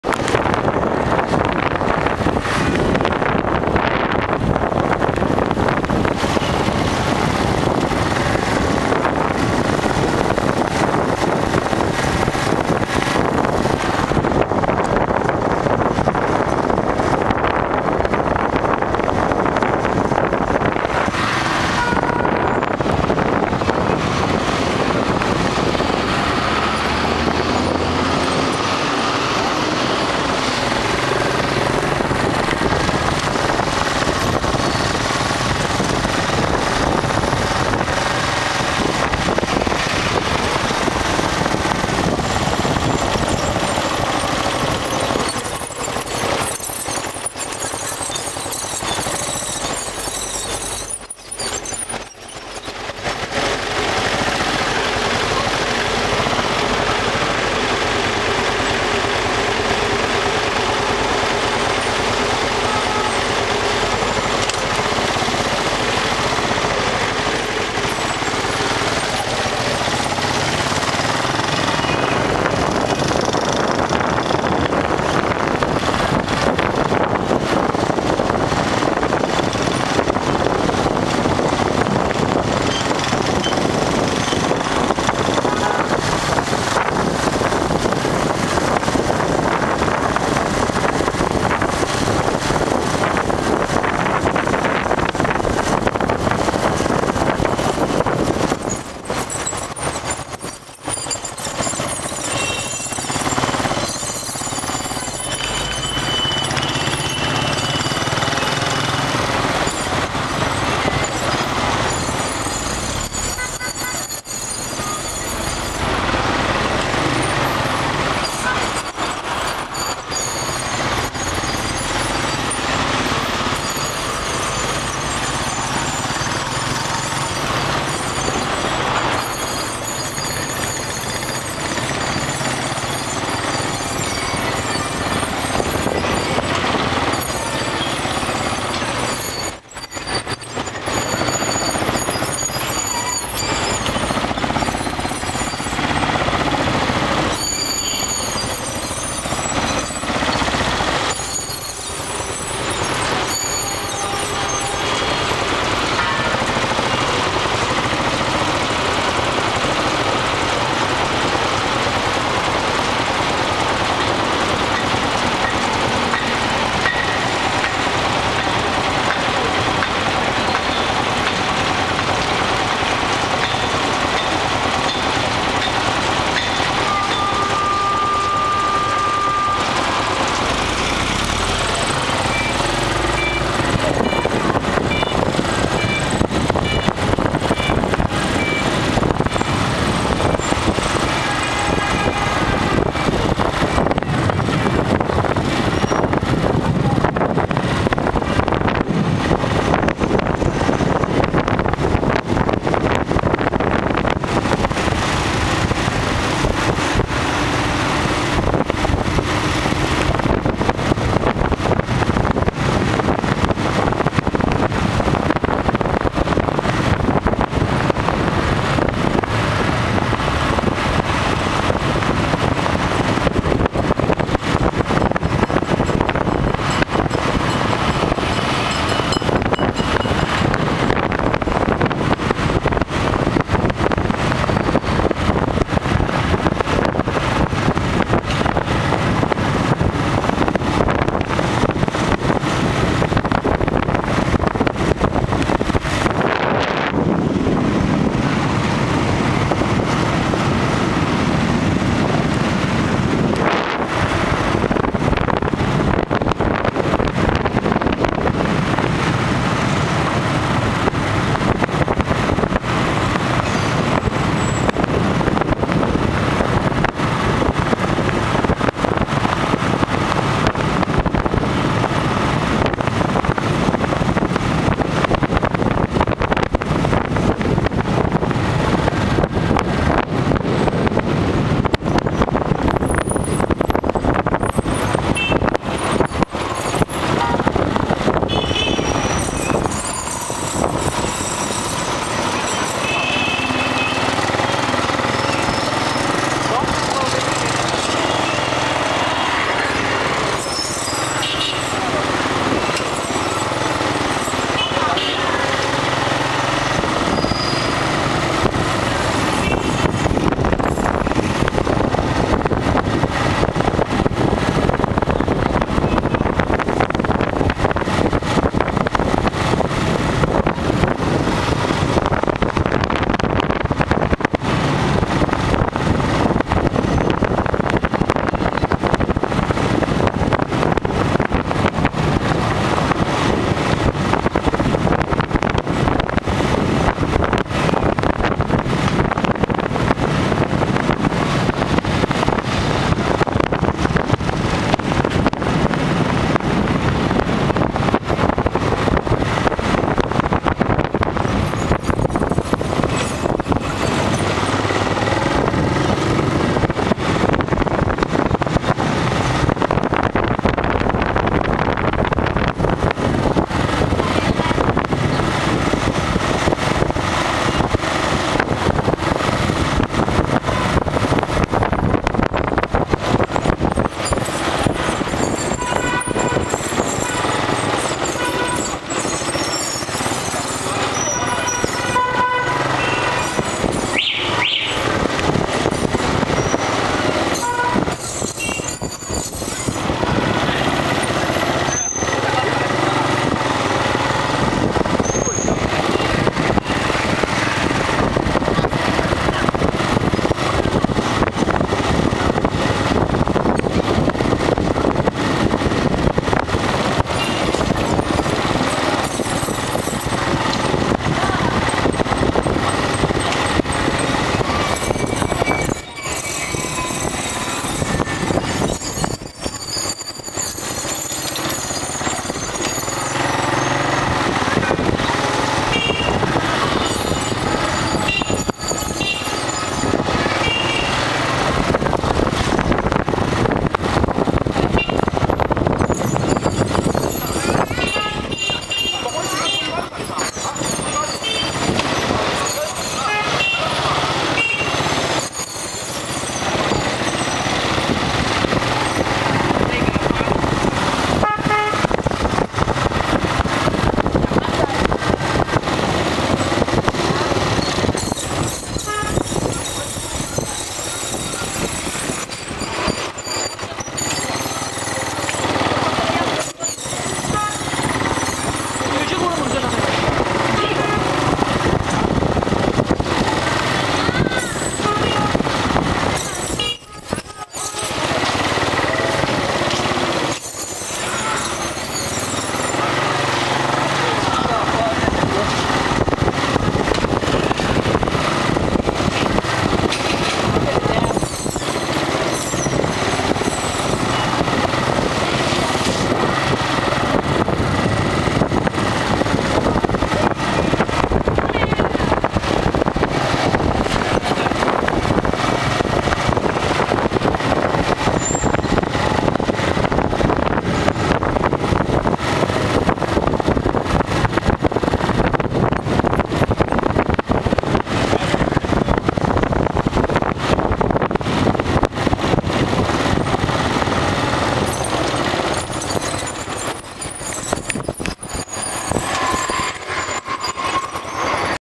Shanghai Motorbike ride
picking up my new tailormade suit with an really cool & fast motorbike taxi...